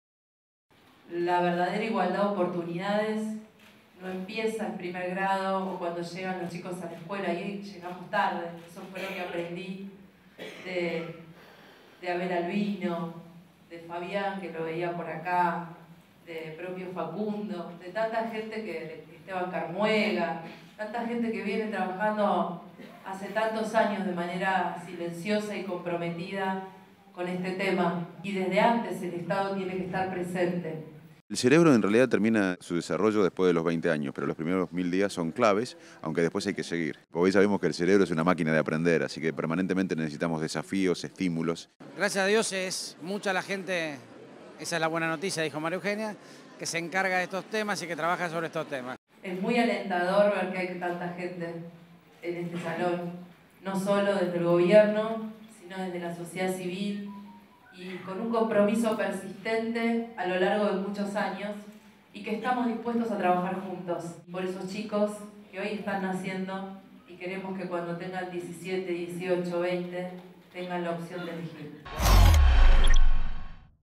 Vidal: "El Estado debe estar presente desde el primer momento"
La gobernadora de la provincia de Buenos Aires, María Eugenia Vidal, encabezó la apertura del Primer Congreso Internacional de Primera Infancia que se realiza en el partido bonaerense de San Miguel.

"Todos quisiéramos que los chicos que nacen hoy y los que van a nacer tengan las mismas oportunidades que tuvieron nuestros hijos", dijo la gobernadora, y agregó que "sabemos que el verdadero camino de fondo para quebrar la pobreza como herencia, se corta desde el embarazo. Lo que hacemos los primeros años es muy importante".

Acompañada por los ministros bonaerenses de Producción, Joaquín de la Torre; y Desarrollo Social, Santiago López Medrano; el intendente local, Jaime Méndez; y por el neurocientífico Facundo Manes, asesor ad honorem a cargo de la Unidad de Desarrollo de Capital Mental, Vidal señaló que "la verdadera igualdad de oportunidades no empieza en primer grado. No podemos llegar tarde, la infancia no empieza a los cinco o seis años sino mucho antes, y el Estado debe estar presente desde ese primer momento".